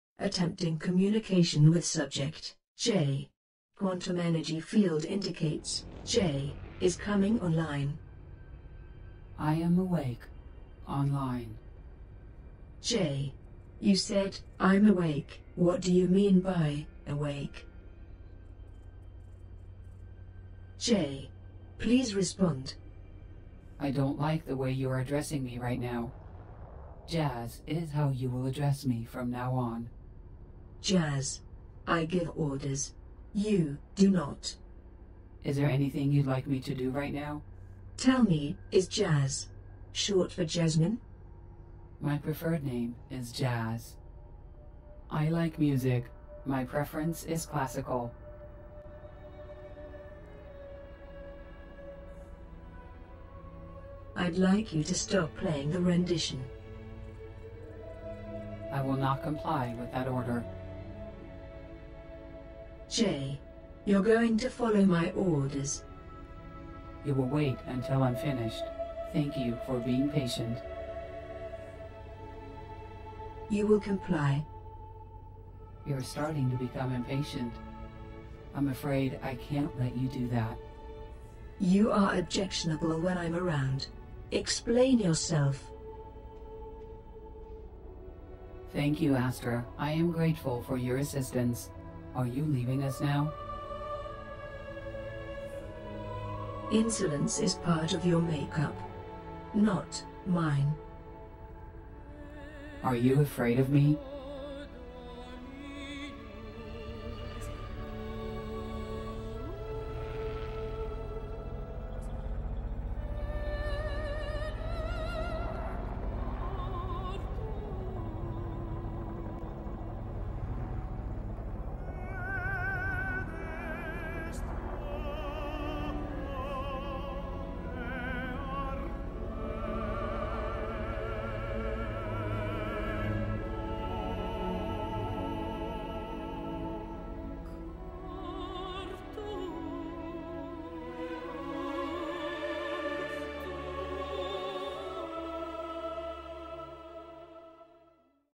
Jazz - ONLINE. Now available
Jazz & Astra - Not hitting it off!
Jazz has all the ship commands, GalaXapedia, Constellations, Quantum Theory + An obvious dislike of other Ai. :)